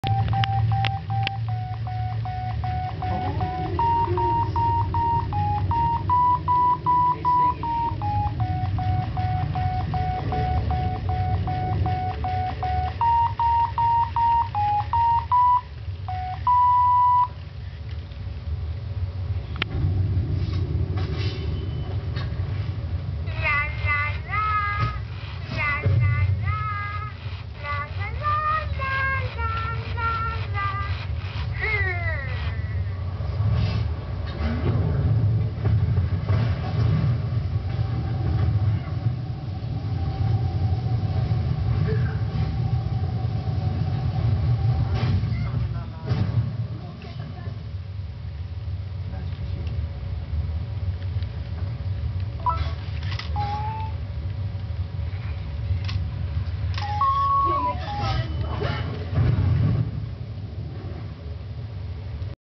Furby is not Impresssed
Atari playing piano for Furby and Furby is not impressed. Poor Atari. Atari shows his displeasure at Furby.